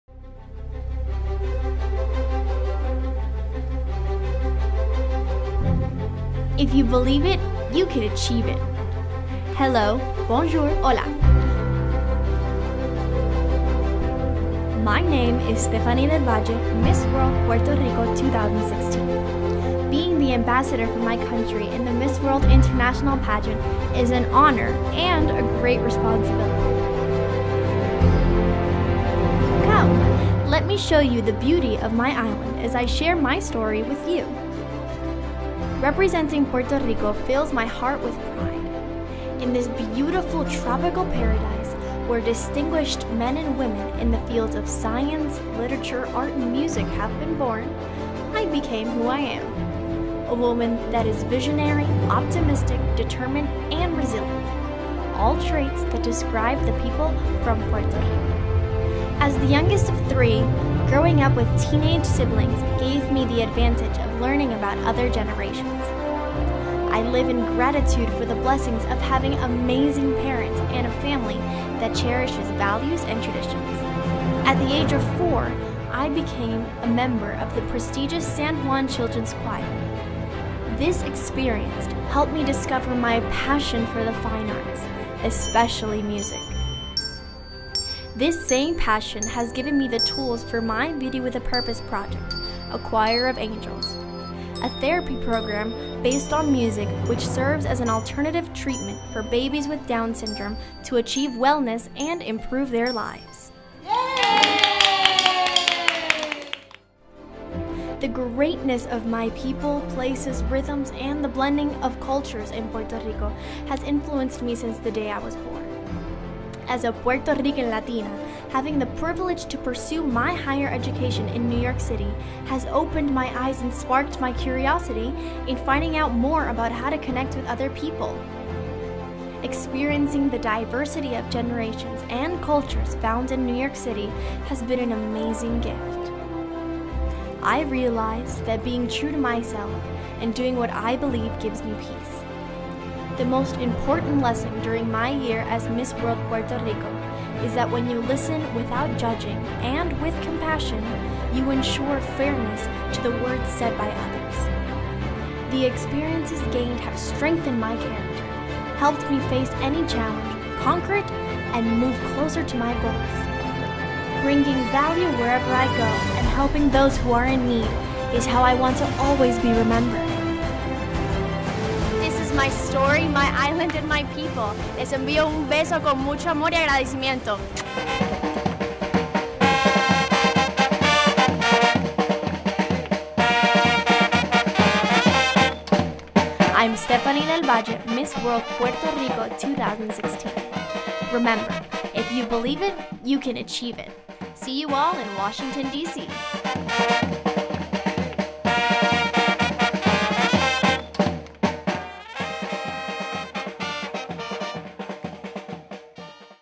PUERTO RICO - Stephanie DEL VALLE- Contestant Introduction: Miss World 2016
Uploaded on 28th oct 2016
Stephanie DEL VALLE, Puerto Rico representative for Miss World 2016!
Miss World 2016 kicks off in November, with the grand final taking place on Sunday 18th December.

• Category
o Entertainment
• Licence
o Standard YouTube Licence
• Category
o Entertainment
• Licence